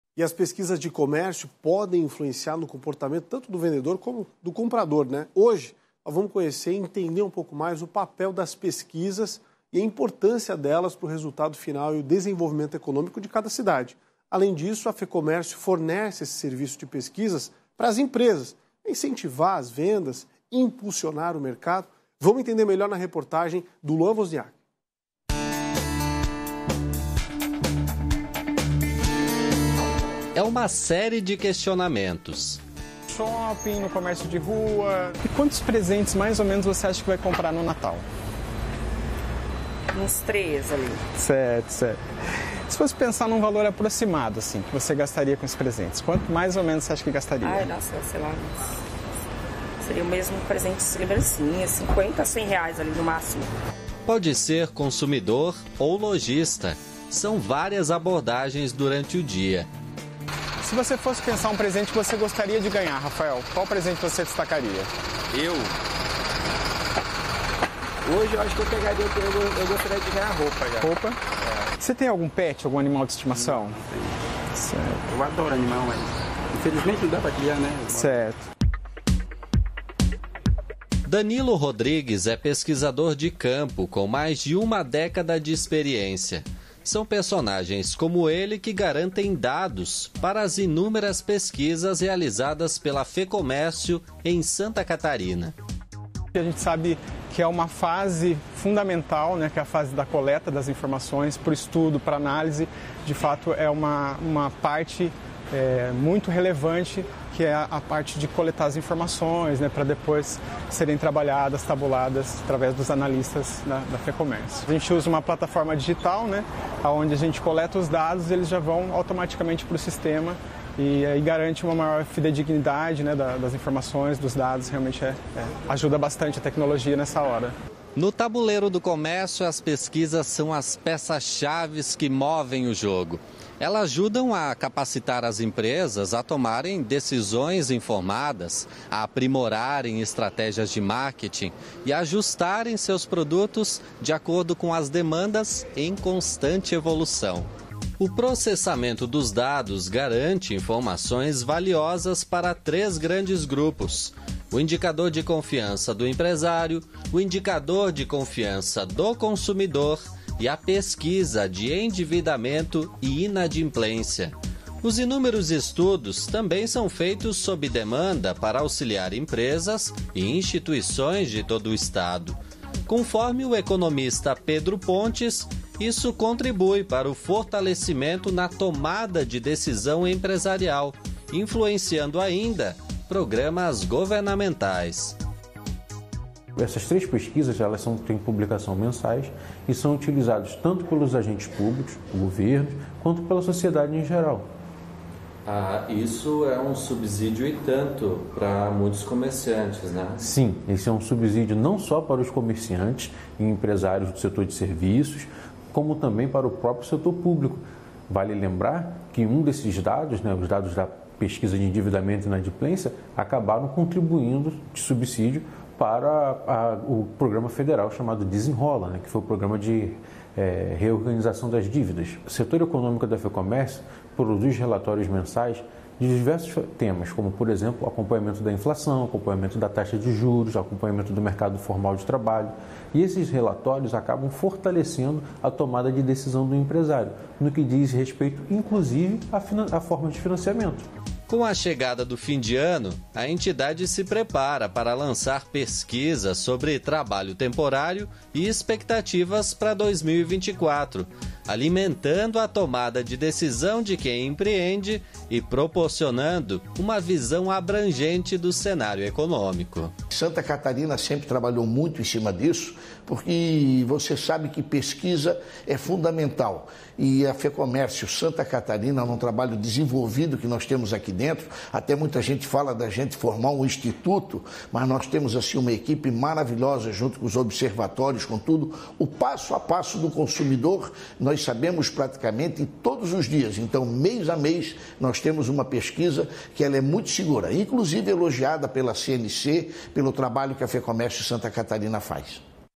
Papel crucial das pesquisas de comércio no desenvolvimento econômico
Descubra como as pesquisas de comércio desempenham um papel fundamental tanto para vendedores quanto para compradores, moldando o comportamento no mercado. Explore a importância dessas pesquisas no resultado final e no desenvolvimento econômico das cidades. Saiba como a Fecomércio está incentivando as vendas e impulsionando o mercado por meio desses estudos.